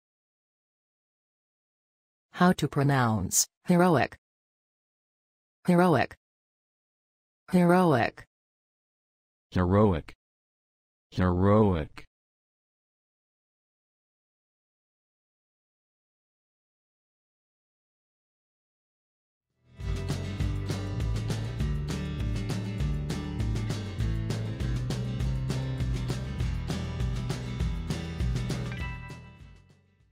Heroic pronunciation
How to pronounce Heroic in English? Listen with us.
What is the correct pronunciation of the word Heroic in everyday English? Speak as the Americans.
How to write the word Heroic? Watch and learn.

Learn how to pronounce Heroic.